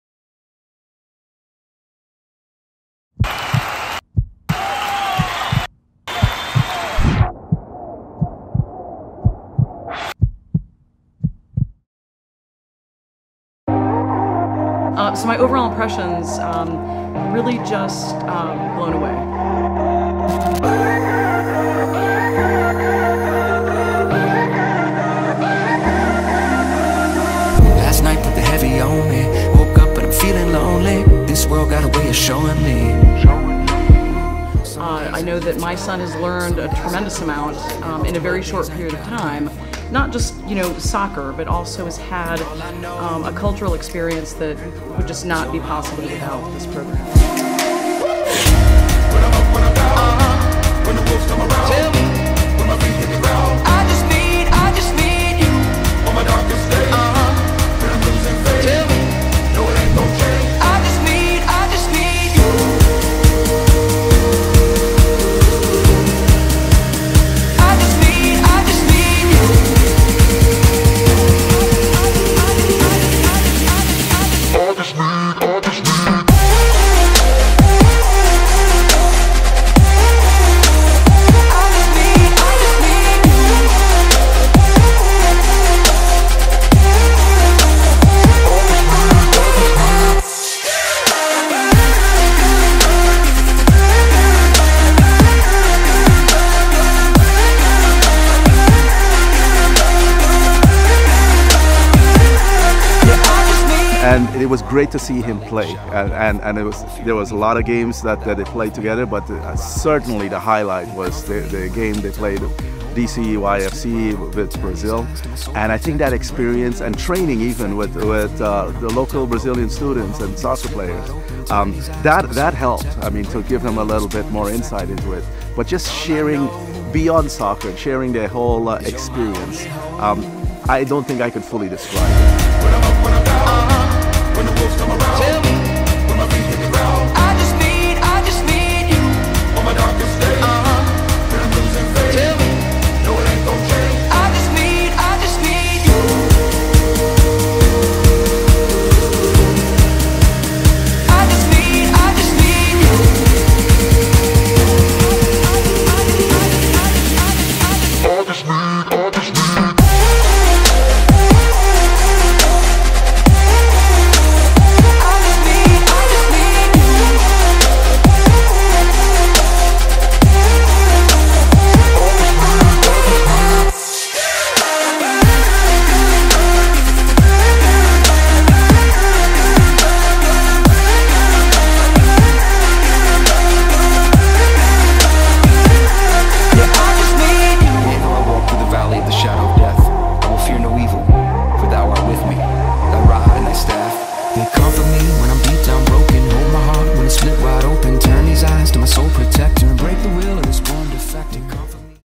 Soccer Camp in Brazil - BR SCORE 2019
Take a look at the best moments of our soccer camp in Brazil! DayDreamMidia has eternalized this EXPERIENCE!